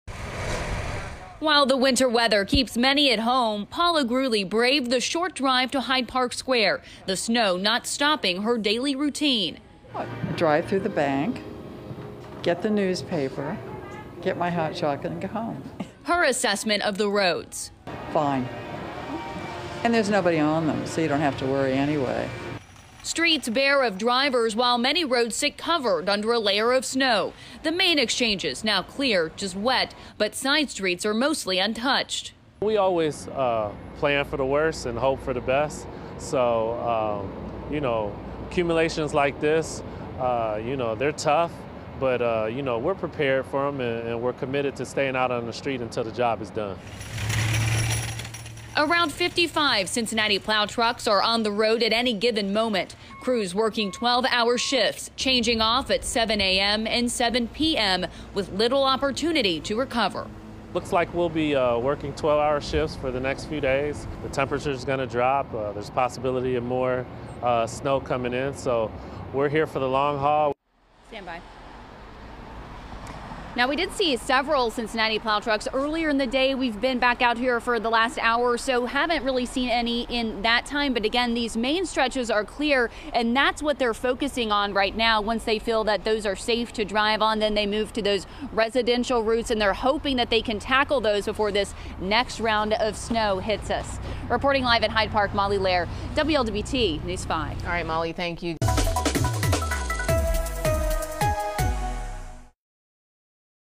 Plow trucks focus on clearing main roads before tackling snowy residential routes
City of Cincinnati crews are working around the clock to plow roads after an overnight snowstorm dropped 8 inches or more on many communities.